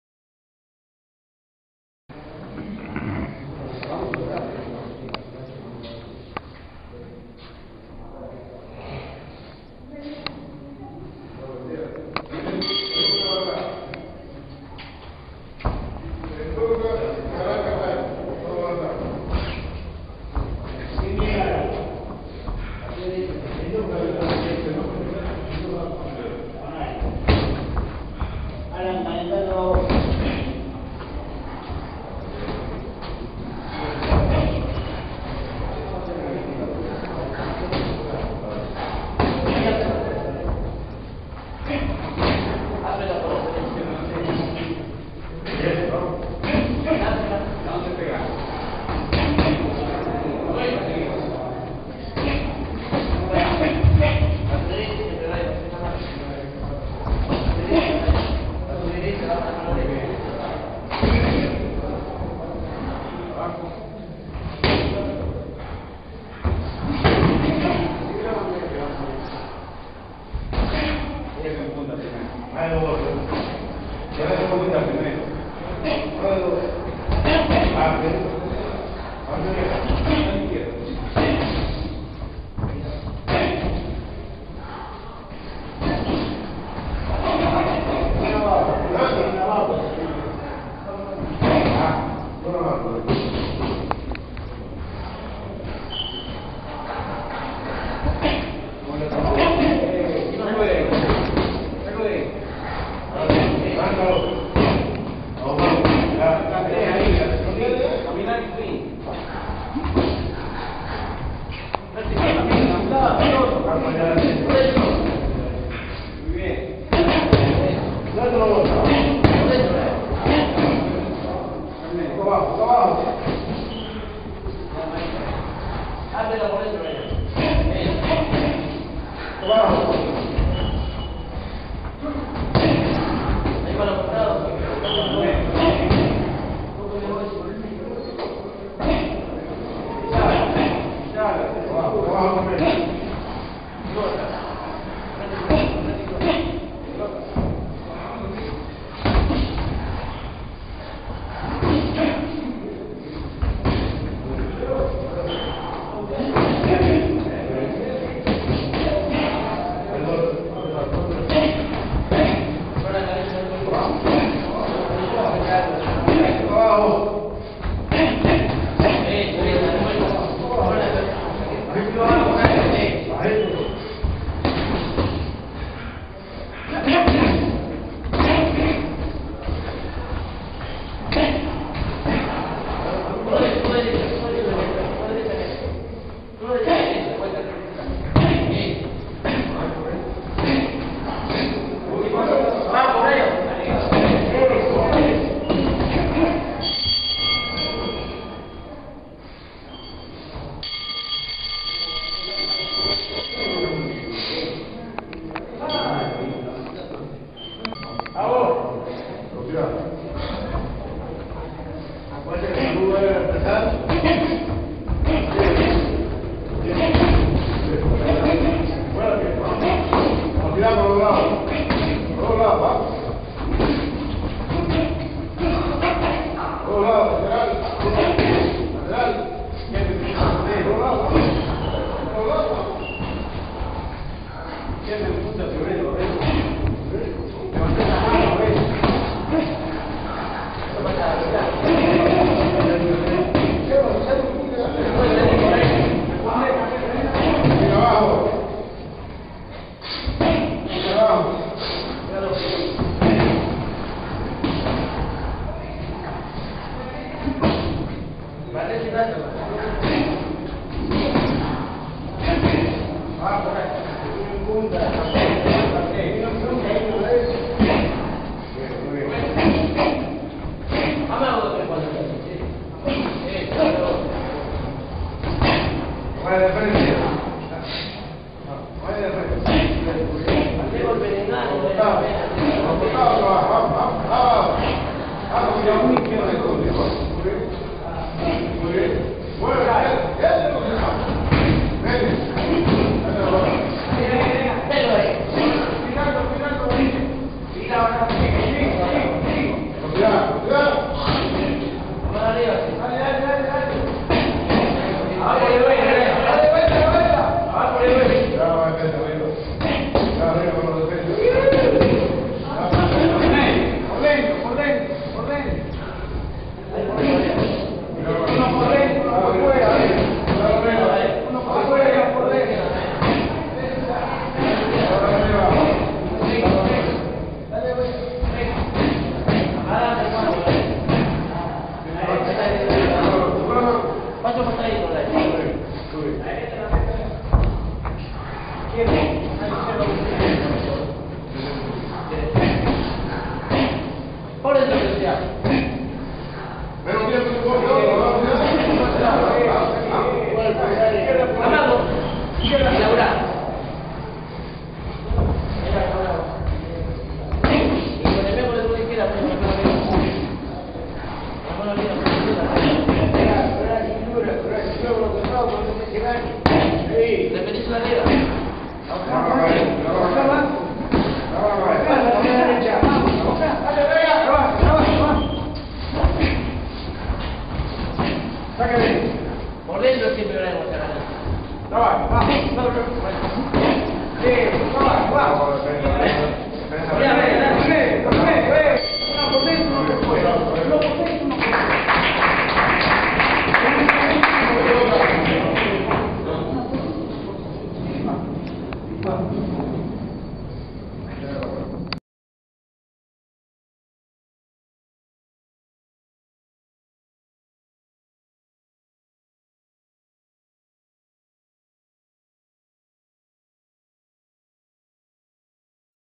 BOX, BRIAN VS ROBLEDO guanteo en el CENARD 26-8-09
S 89), A 77) BOX ARGENTINO CAMPEONES disfrutando de la maestria DE NUESTRO genial seleccionado para el mundial de ITALIA, ANTE UN IMPECABLE ROBLEDO Boxeo amateur,buitre argentina, campeon nacional,buitres internacional club excelente nivel, argentine champion boxing,cenard , ENTRENAMIENTO, FAVORITOS